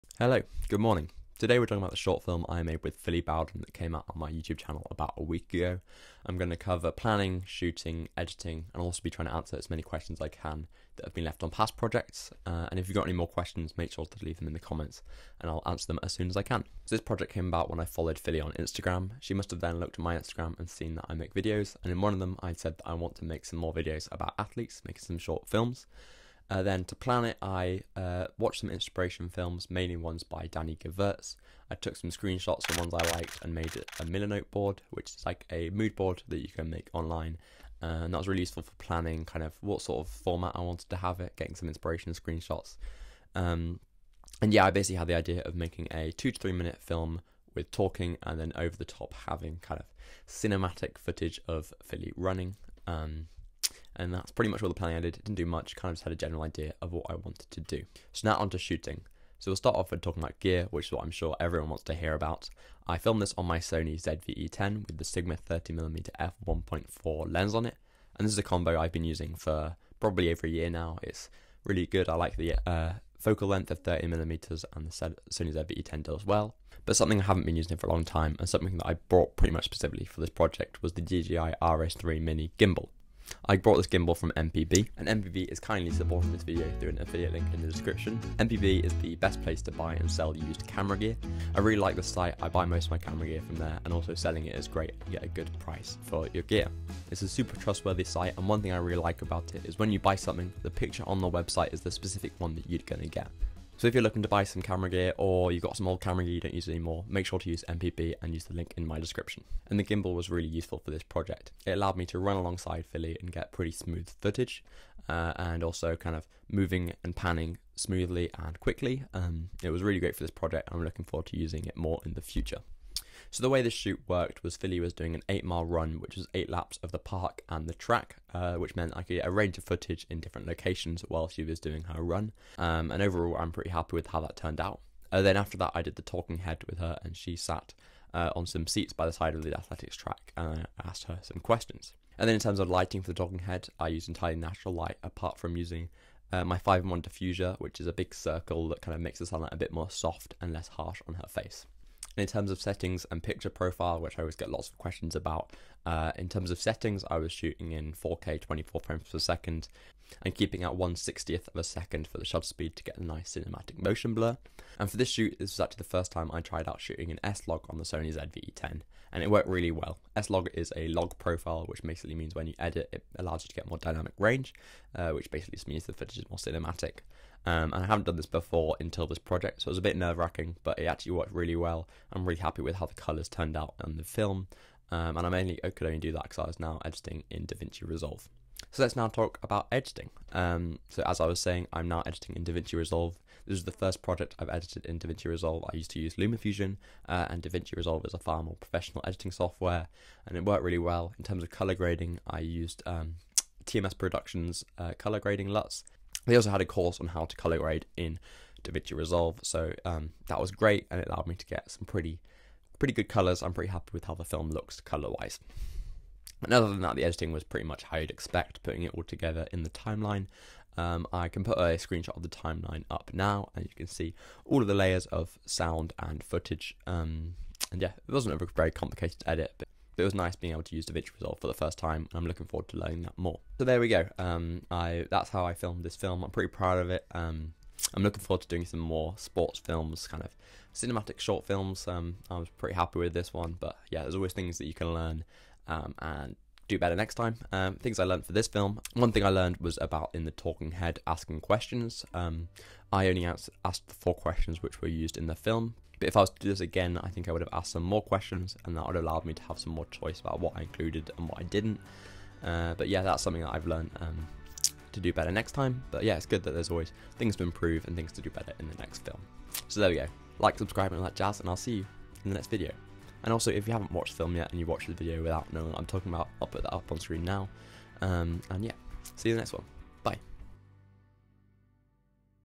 How I Filmed a Sports short film on the SONY ZV-E10 - Planning, Shooting, Editing
MPB.com, the best place to buy and sell used camera gear